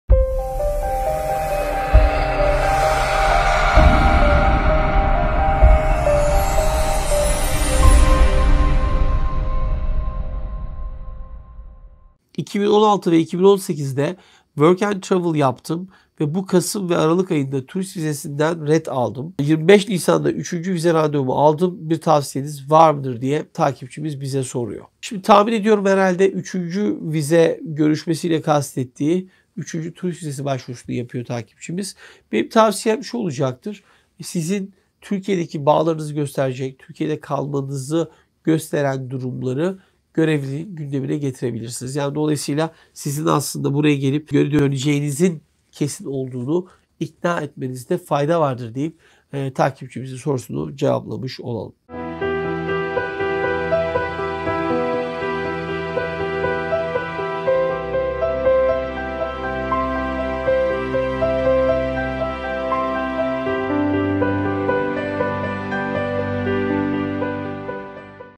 WORK AND TRAVEL SONRASI TÜRKİYE’DEN ABD VİZESİ ALACAKLARA TAVSİYENİZ NEDİR ?
Soru: 2016 ve 2018’de work and travel yaptım ve bu kasım ve aralık ayında turist vizesinden ret aldım. 25 Nisanda 3. vize randevumu aldım. Bir tavsiyeniz var mıdır ?